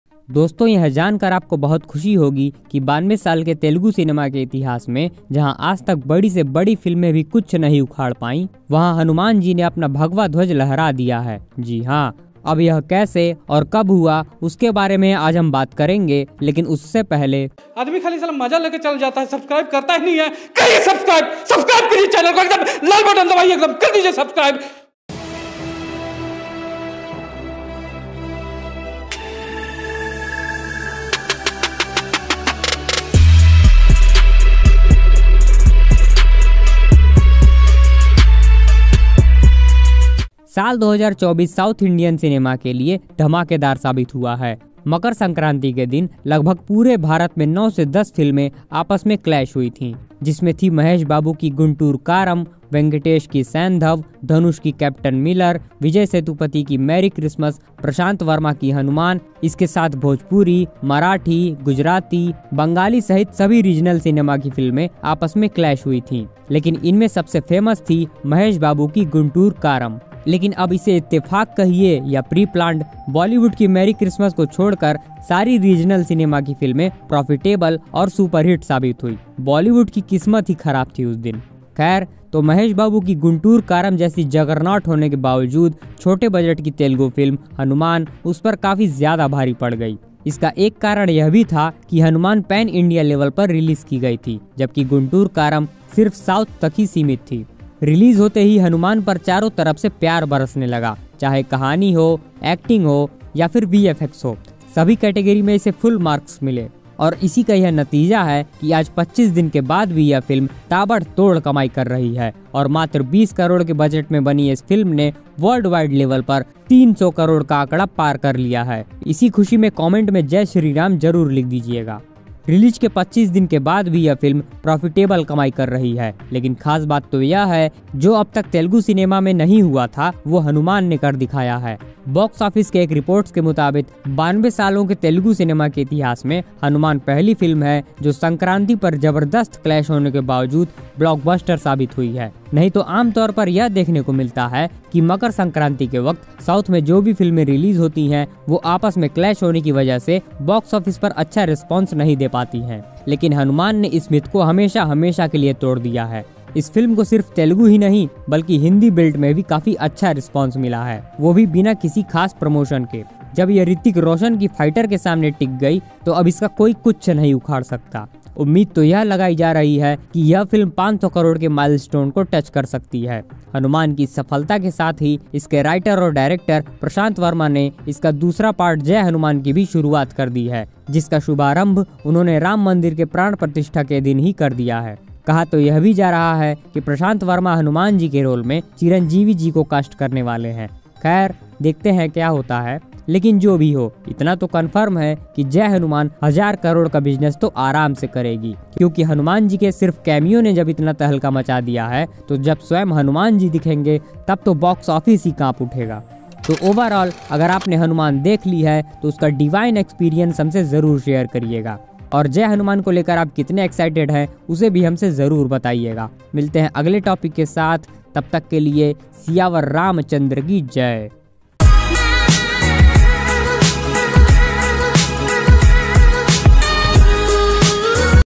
Hanuman - Box Office Collection & Discussion | Prasanth Varma | Teja Sajja | Review Studios
A Superhero film of Akhand Bharat, Written & Directed by Prasanth Varma and produced by Primeshow Entertainment. Starrring Teja Sajja in the title role, alongside Amritha Aiyer, Varalaxmi Sarathkumar, Samuthirakani, Vinay Rai and Vennela Kishore. The film is set in the fictional village of Anjanadri and is the first installment of the Prasanth Varma Cinematic Universe (PVCU).

Scripted & Narrated By : Ankit Srivastava

For More Plzzz...
"Review Studios"